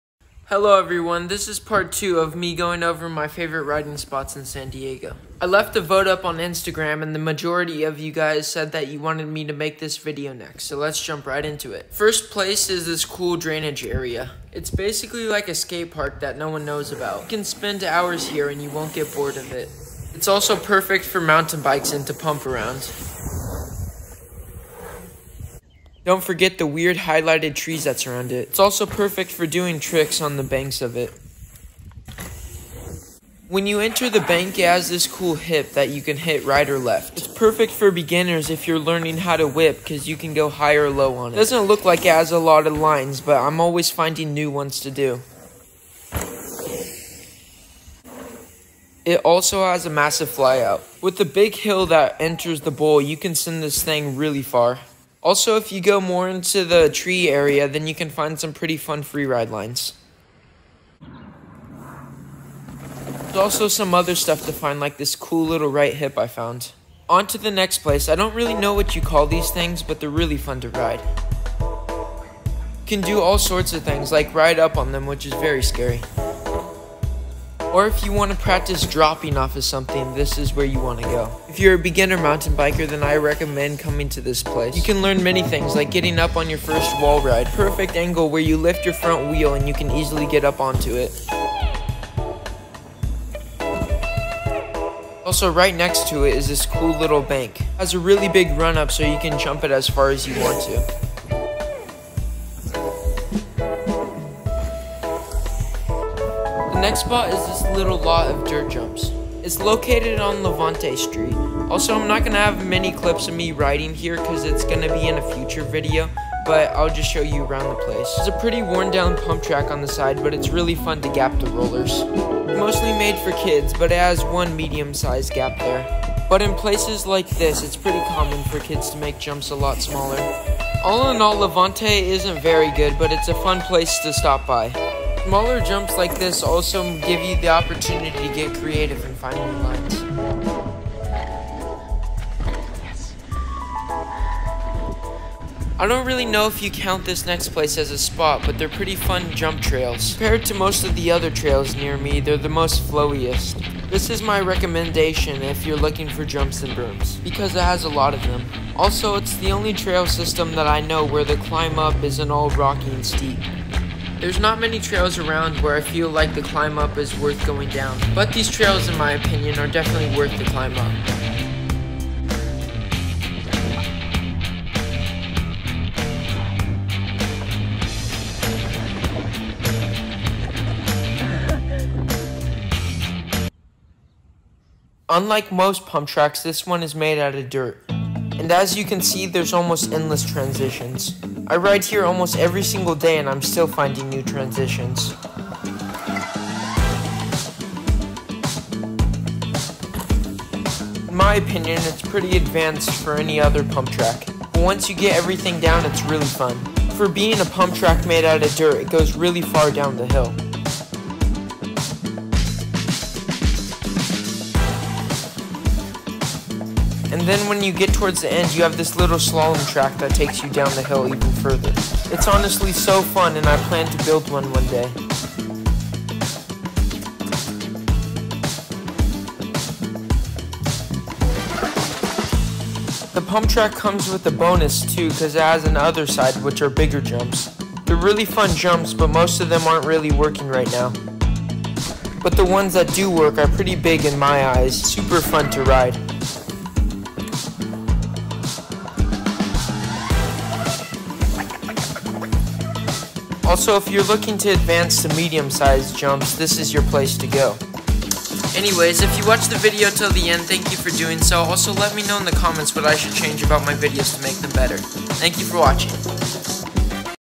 MY FAVORITE RIDING SPOTS IN SAN DIEGO PT.2
This is part 2 of me going over my favorite riding spots in San Diego. Pt. 3 is coming soon :)